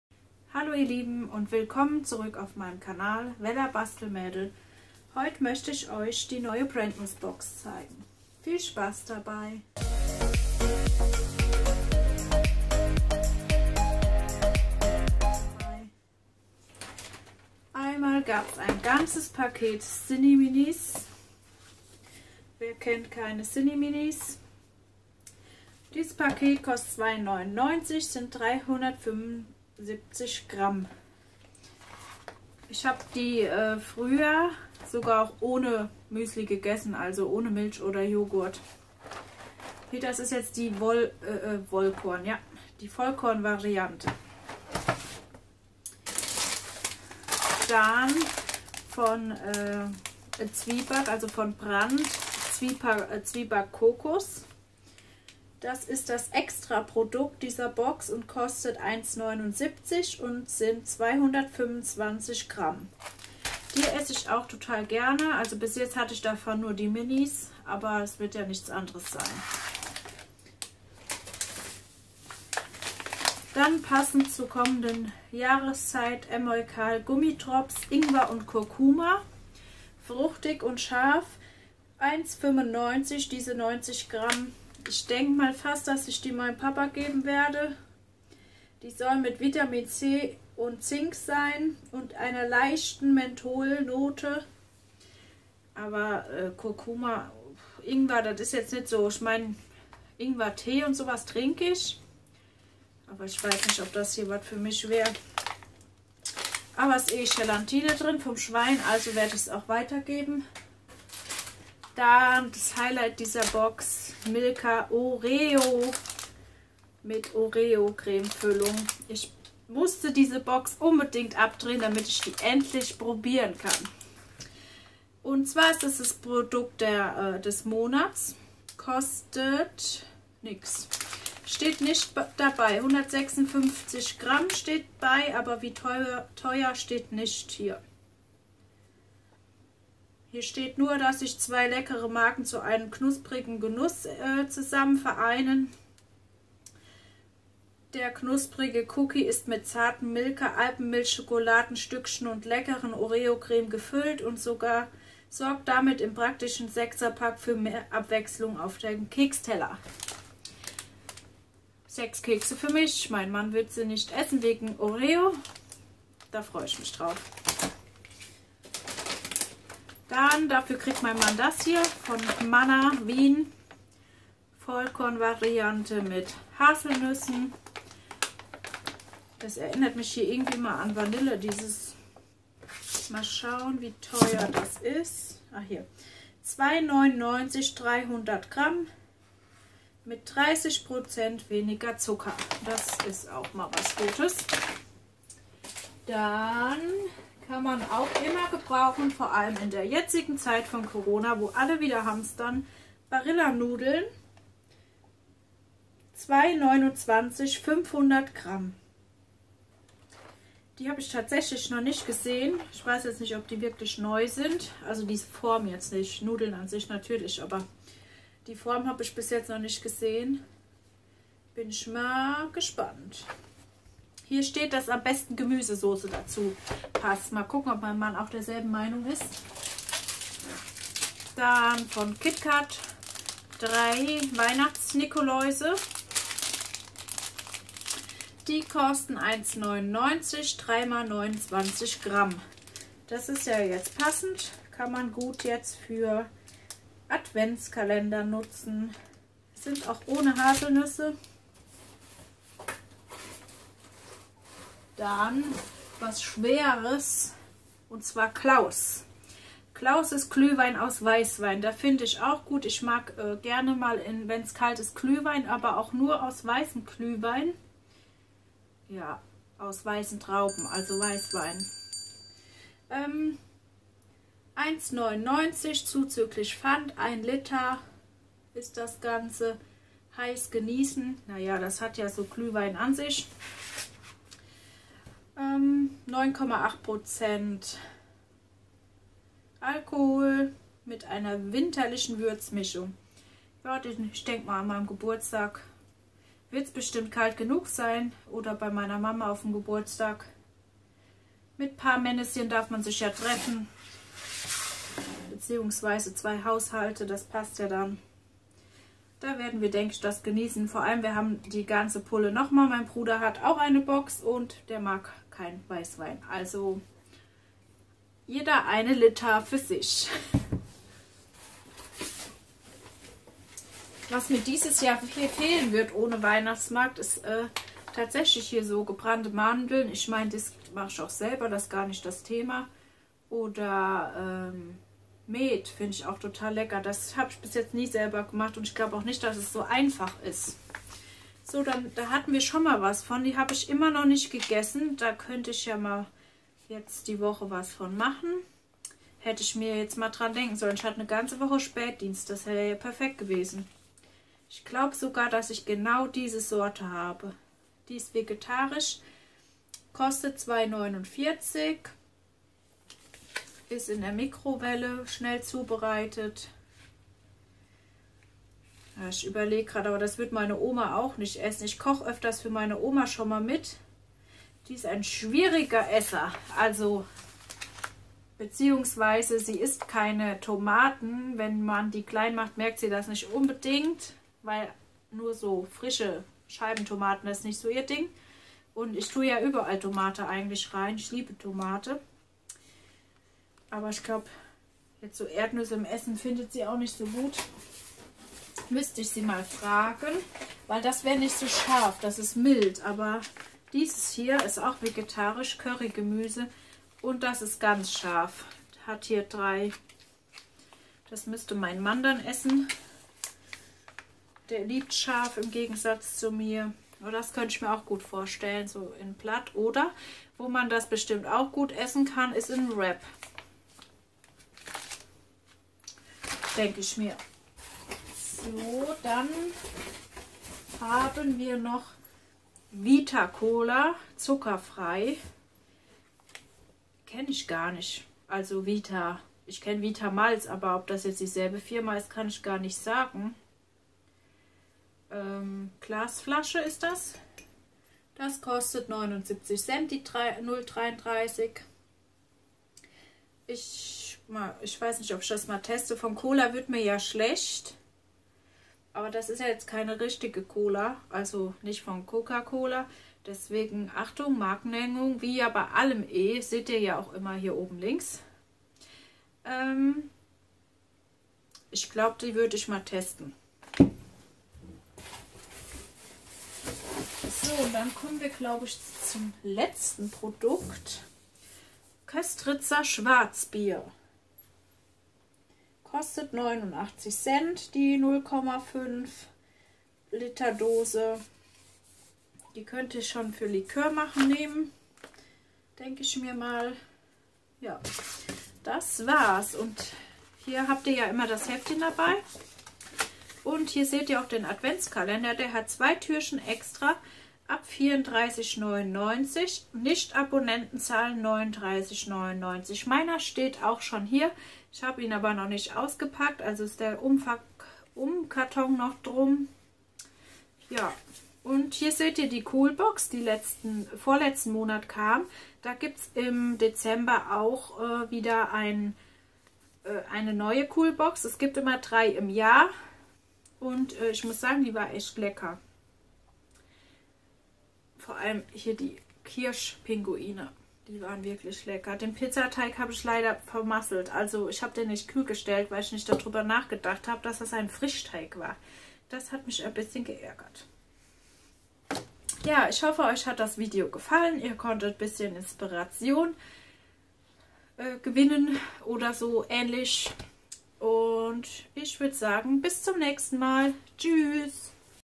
Brandnooz-Box September
Huhu, meine Lieben, heute zeige ich euch wieder eine Brandnooz-Box:-)